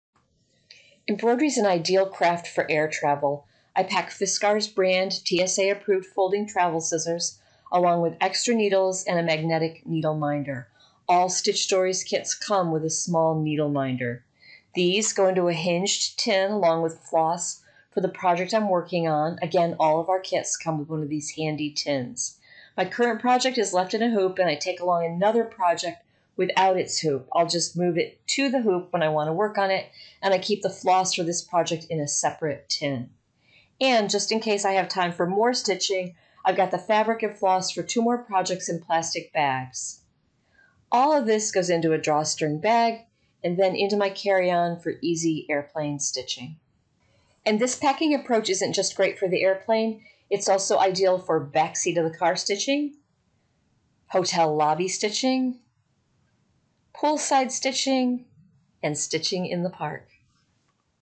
How to Pack Your Embroidery Projects for Travel
Embroidery is an ideal craft for your air travel and vacations. It’s lightweight, doesn’t take up much space in your bag and doable in the room of your seat tray. Watch this one-minute video, to see how I pack my embroidery for a carry-on-only trip.